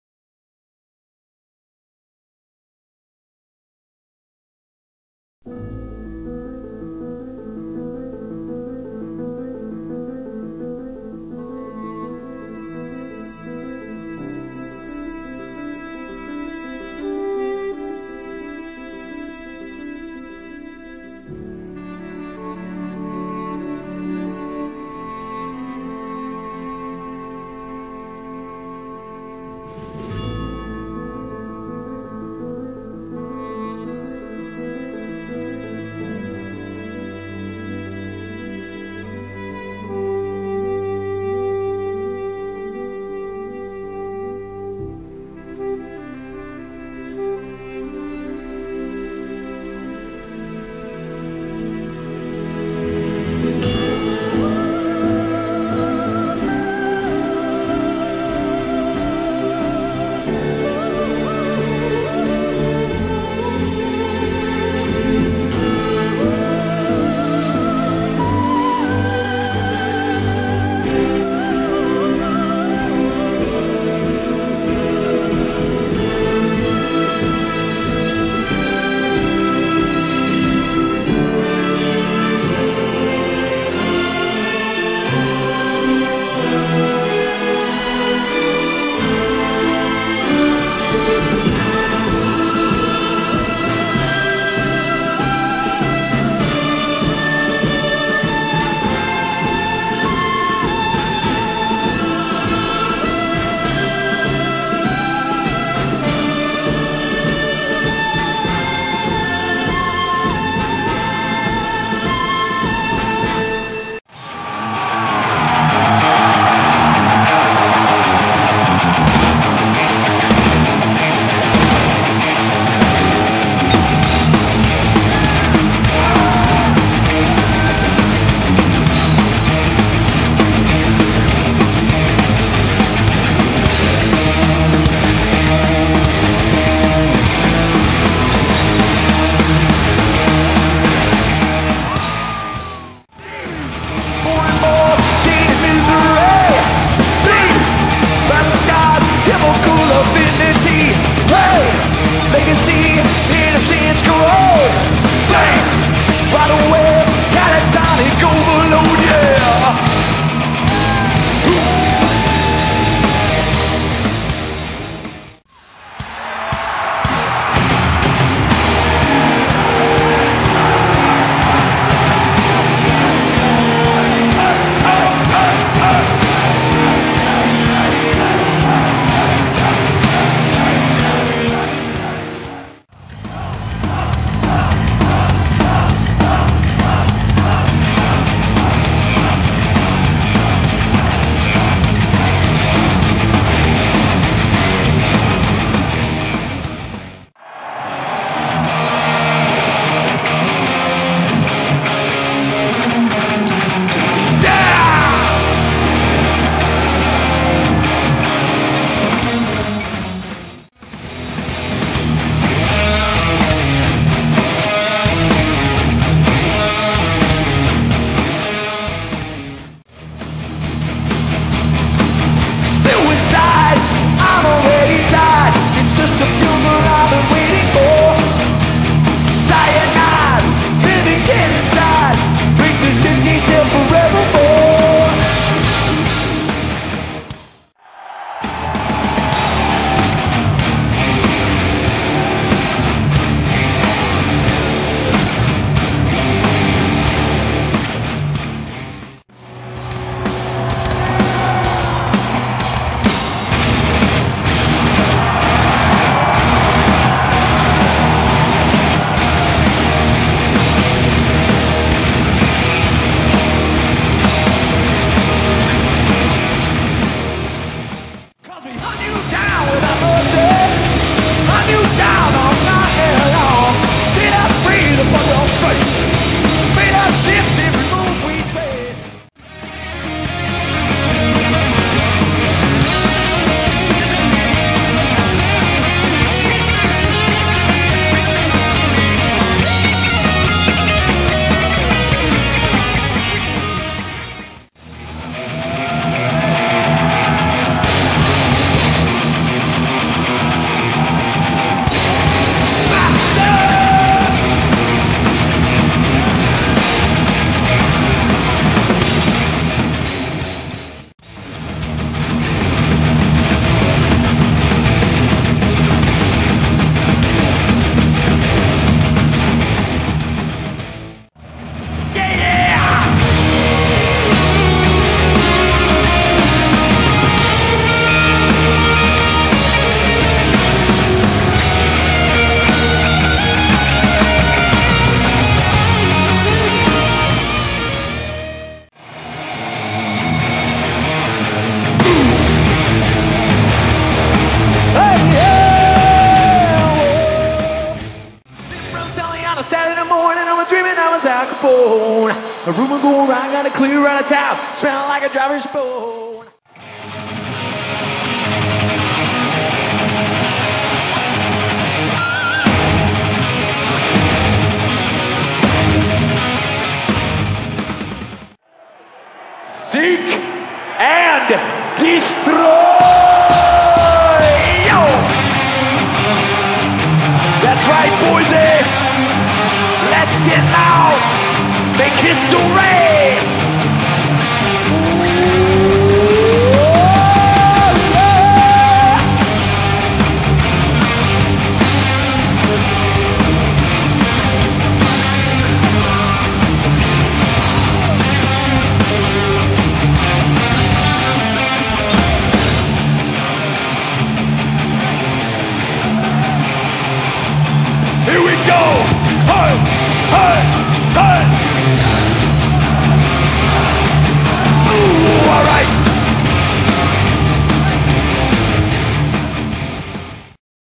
Metallica Concert - Boise
A slide show with pics we took and some pics from metontour.com.  I took clips of every song from that show and put them to the pics, in some cases even matching the pics up with the song they were taken during.  Enjoy.

Boise, ID - December 7th, 2009